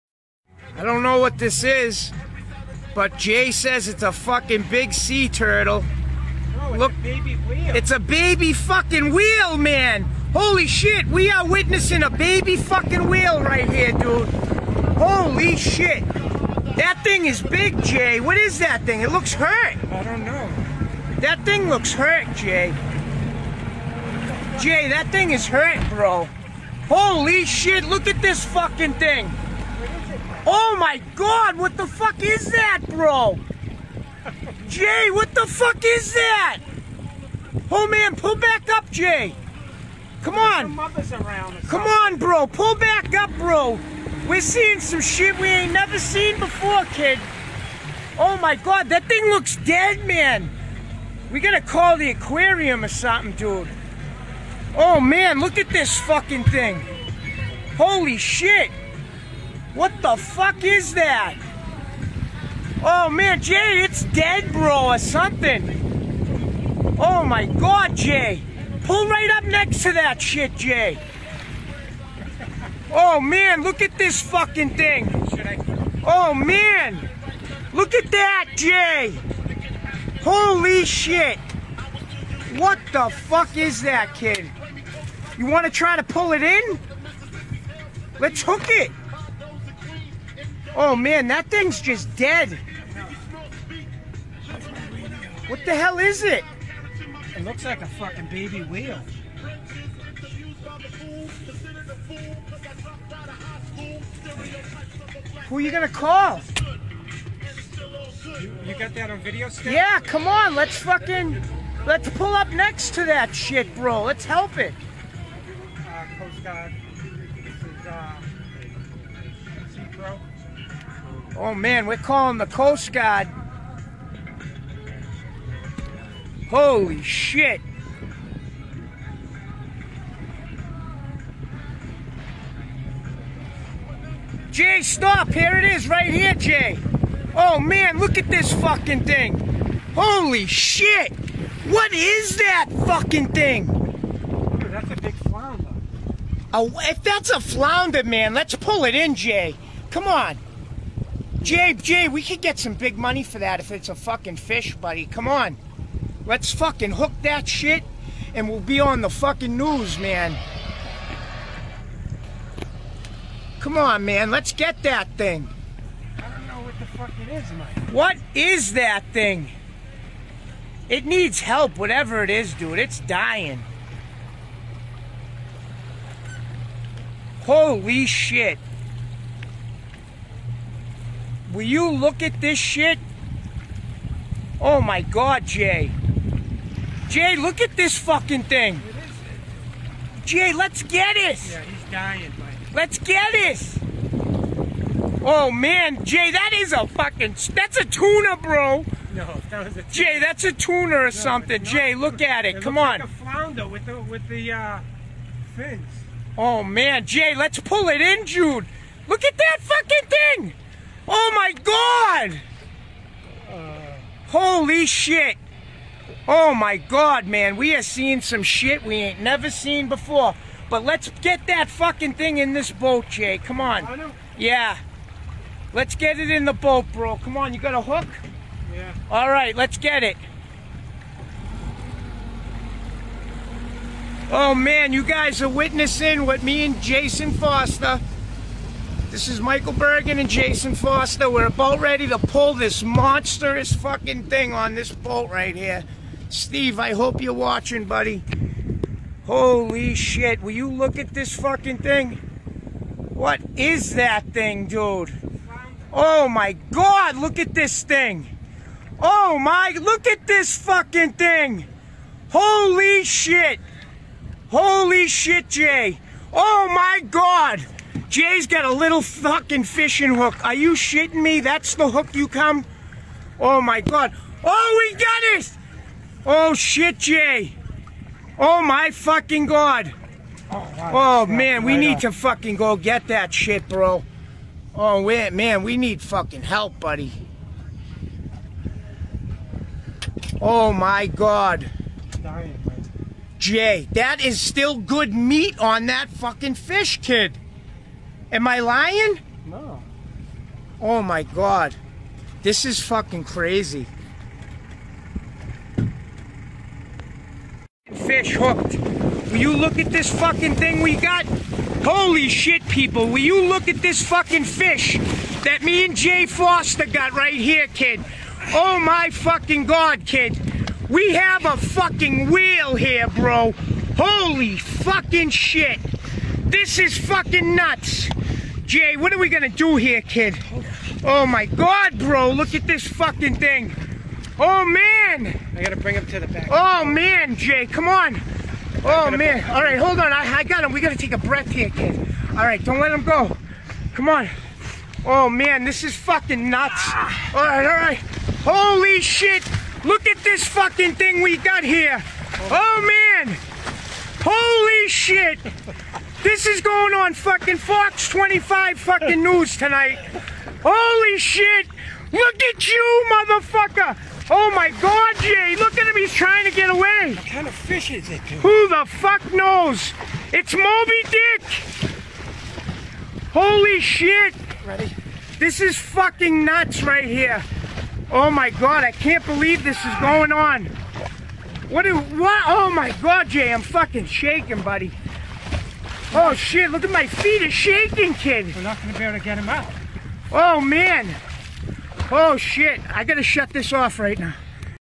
"We're seeing some shit we ain't nevah seen befoah kid"! (longer version)
Two Bawston fucking retahds catch a baby fucking wheel, or is it a tuner?